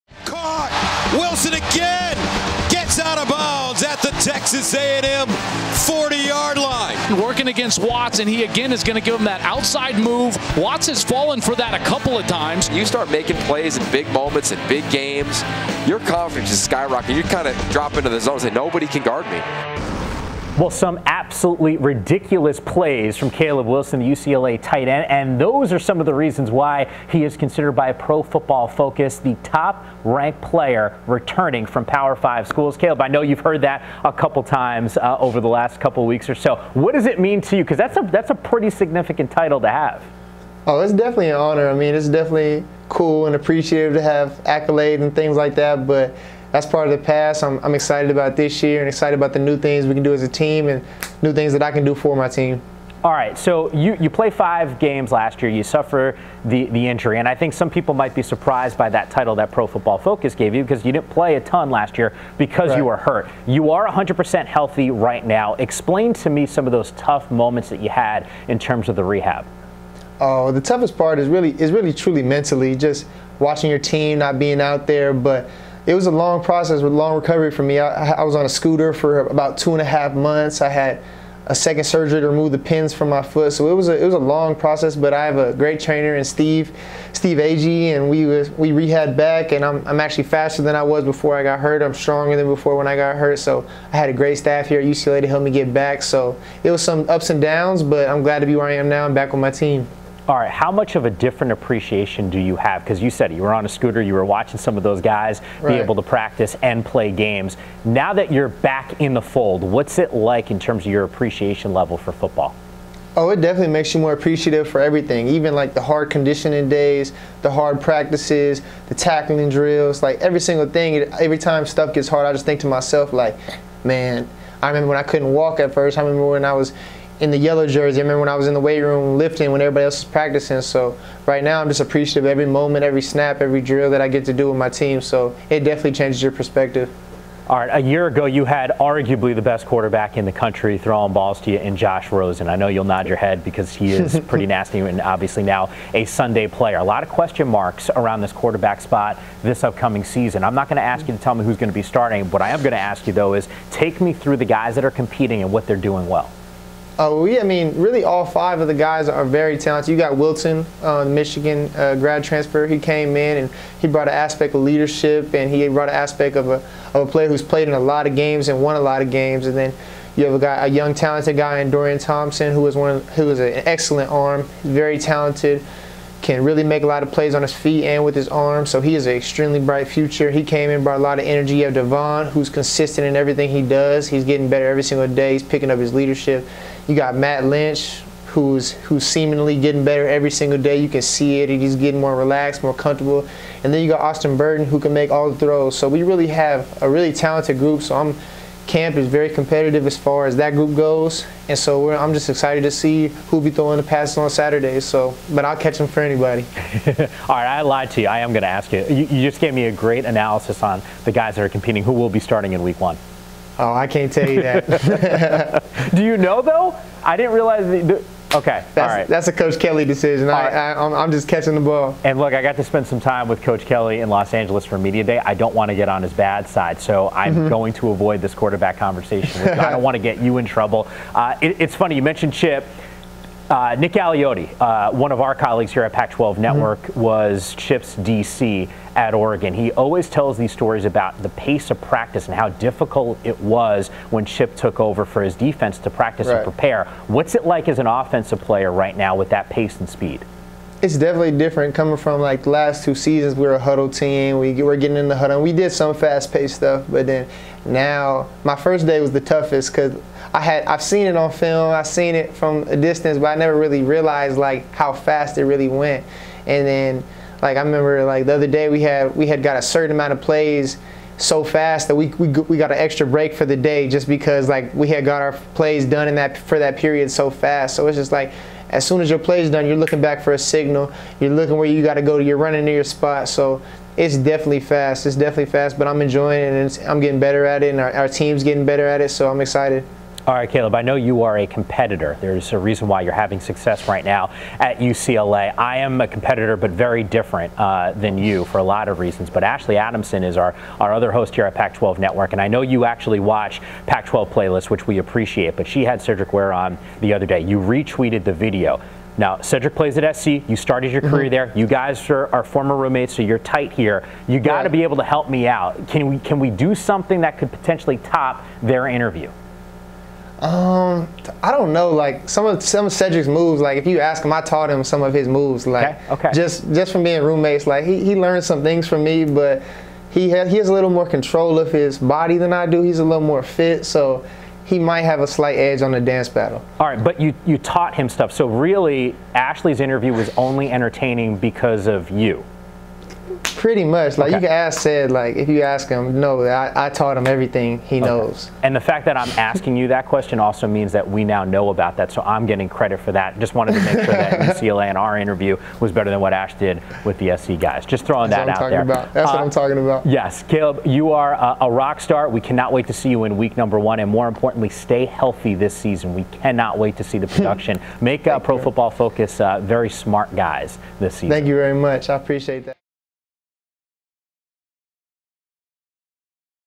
UCLA's Caleb Wilson on high expectations heading into Chip Kelly era
Pac-12 Networks' Mike Yam chats with UCLA's Caleb Wilson about his expectations heading into the Chip Kelly era this season in Westwood. The redshirt junior tight end also talks about how different practices are in their new uptempo offense.